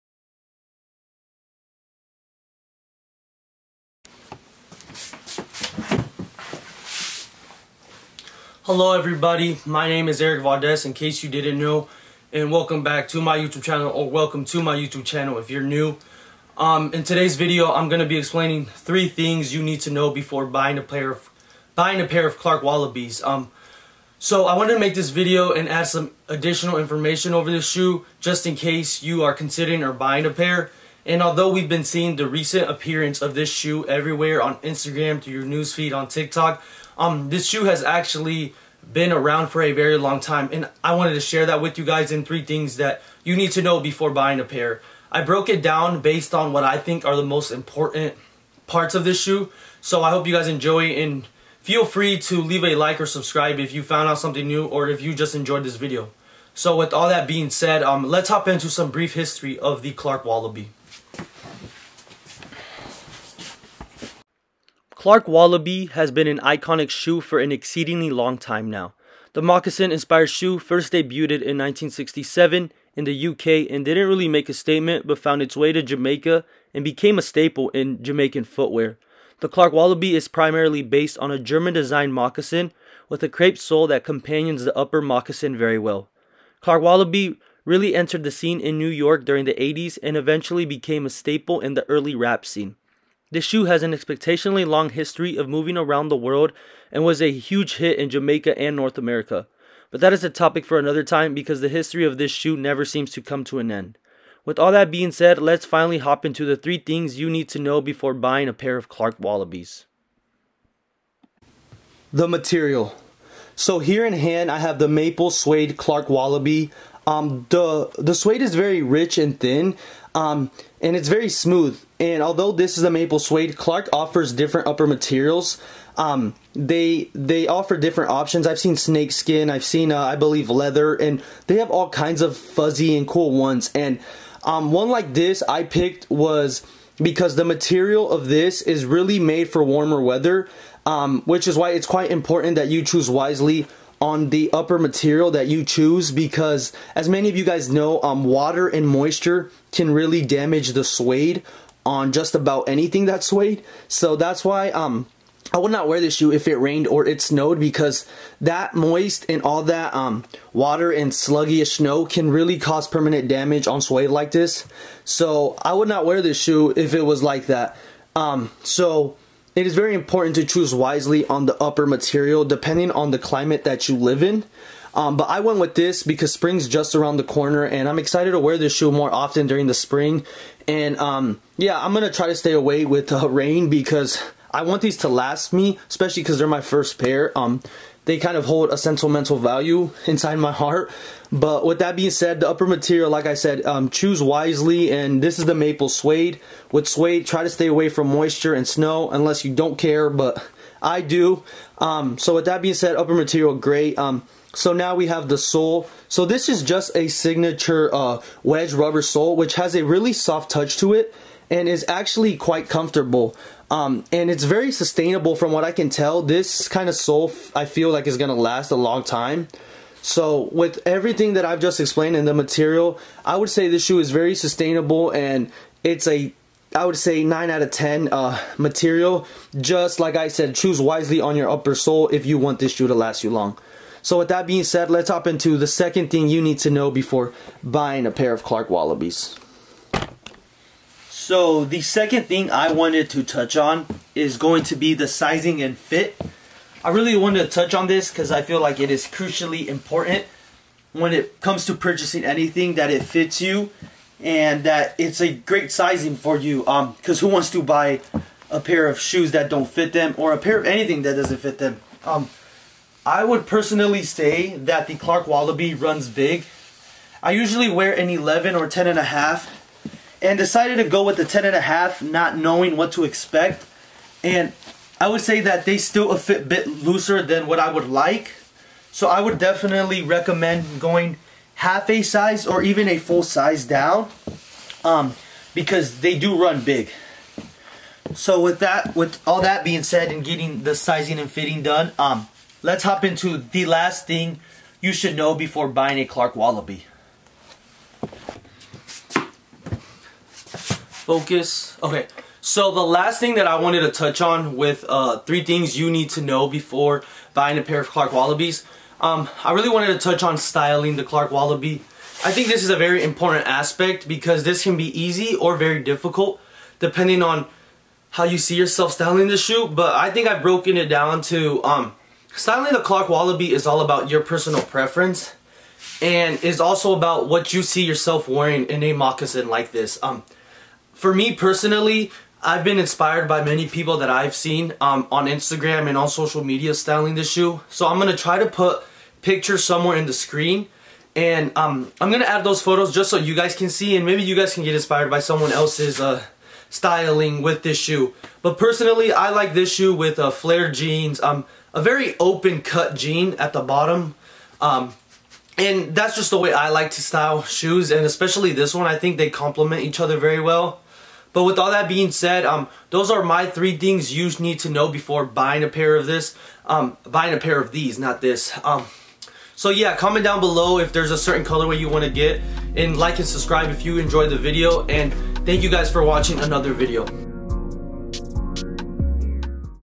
3 things YOU NEED to know before buying The Clarke Wallabee
This video is based on the Clarke Wallabee and 3 things you need to know before buying.